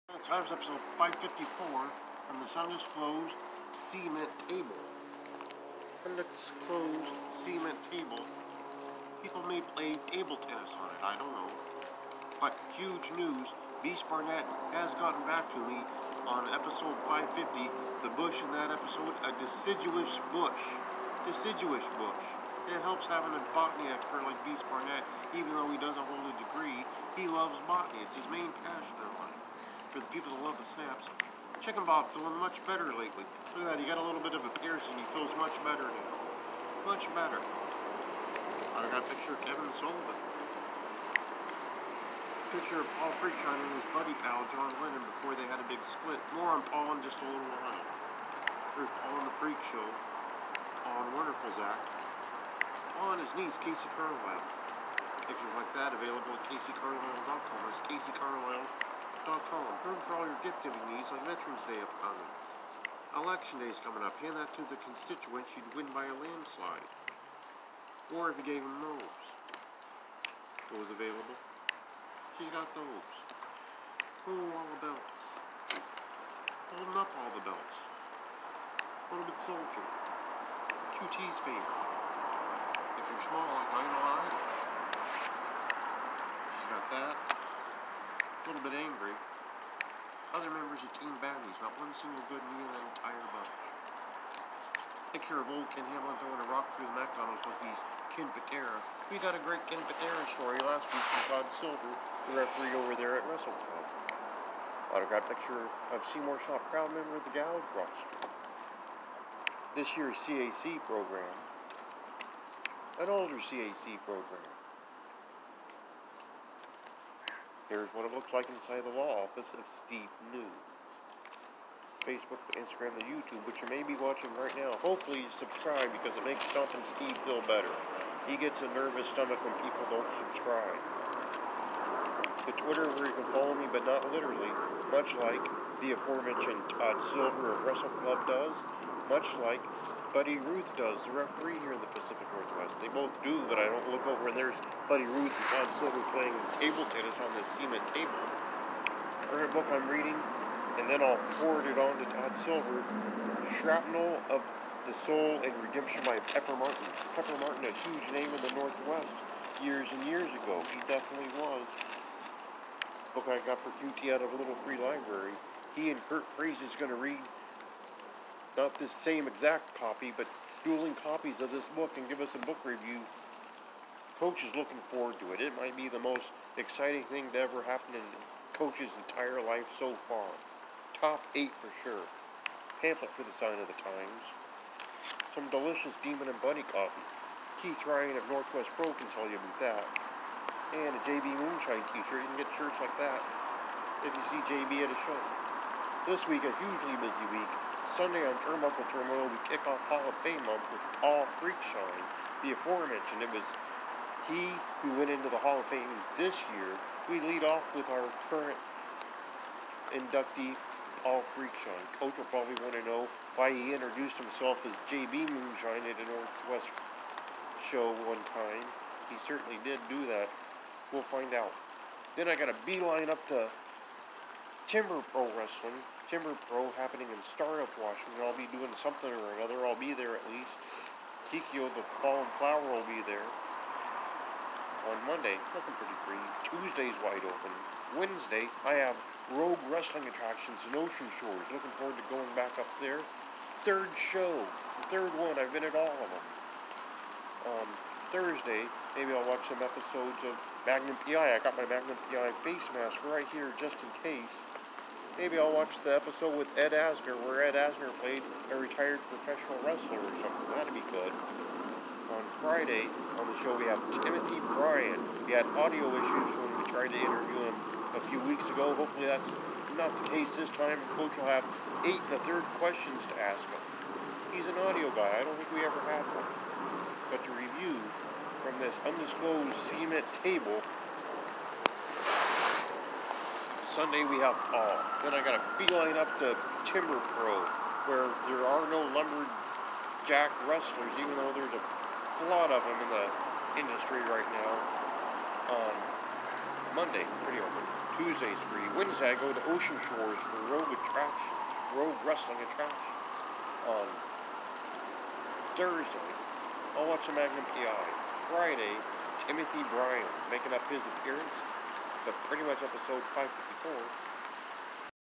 The Sign of the Times Episdoe 554 (The Chicken Bob Feeling Better Edition)
From an undisclosed cement table comes episode 554 of The Sign of the Times. Chicken Bob is feeling much better lately and is ready to find out about the busy week that Sign Guy has ahead of him. There is also a major botany related update.